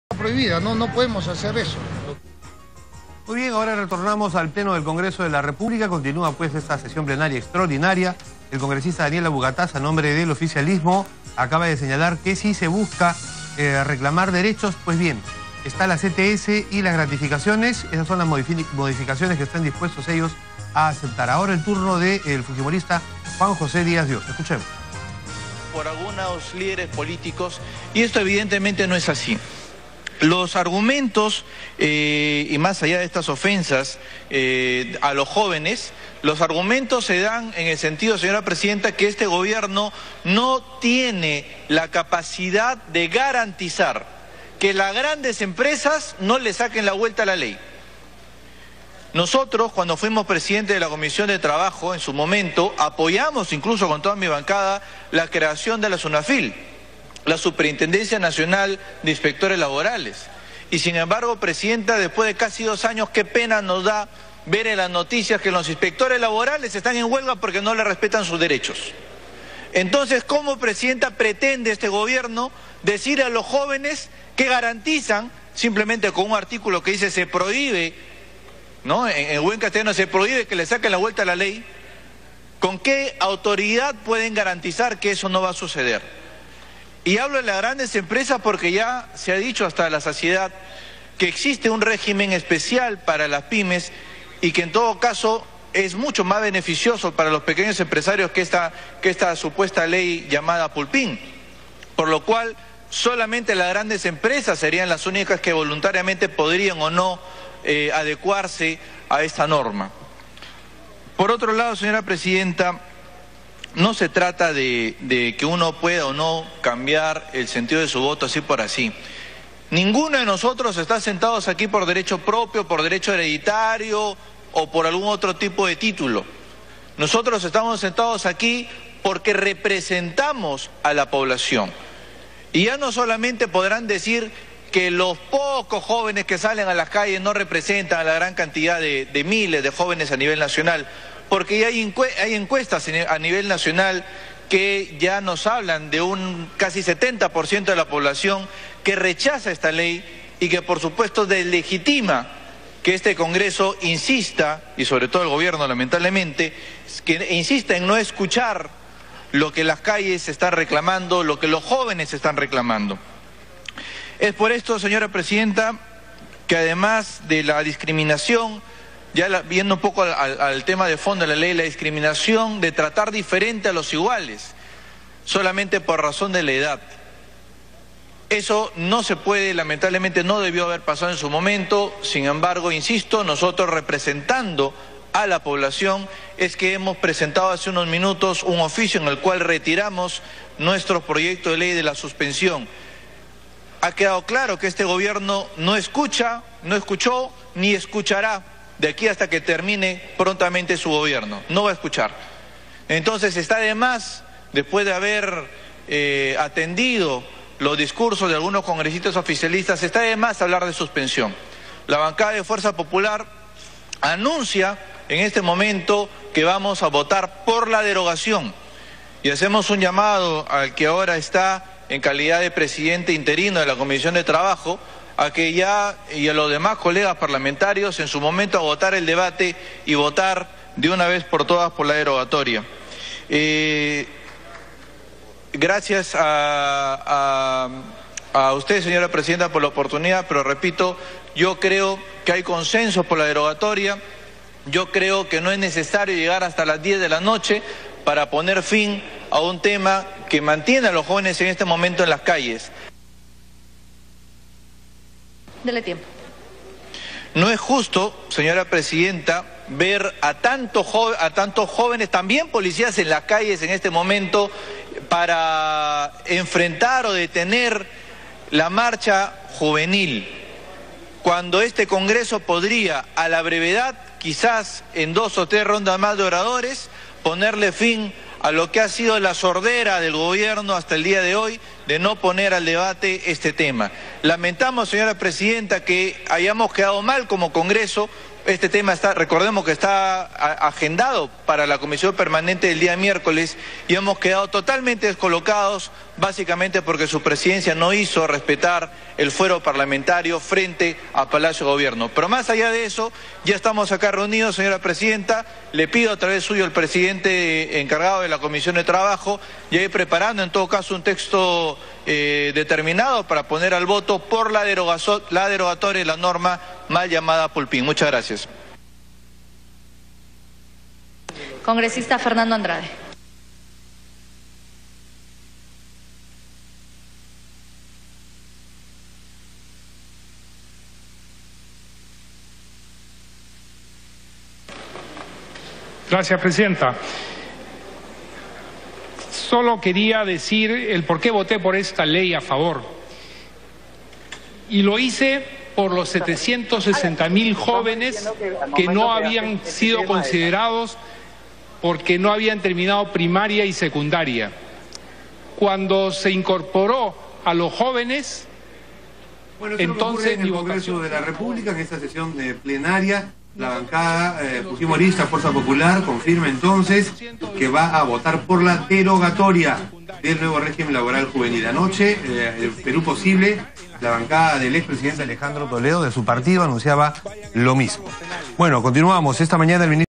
Participación de Juan José Díaz Dios en el Congreso
26/01/2015 - El legislador Juan José Díaz Dios hizo referencia al tipo de gestión de la Sunafil lamentando que el Parlamento ni el gobierno escuchen las opiniones de la población. En ese aspecto, informó sobre el retiro del proyecto de ley de suspensión y anunció que la bancada de Fuerza Popular votará por la derogación de la nueva norma laboral juvenil.